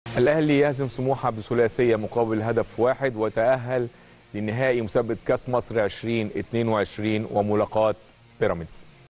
أيمن شوقي: الأهلي يهزم سموحة ويتأهل لنهائي كأس مصر لملاقاة بيراميدز
أيمن شوقي: الأهلي يهزم سموحة ويتأهل لنهائي كأس مصر لملاقاة بيراميدز | الأستوديو التحليلي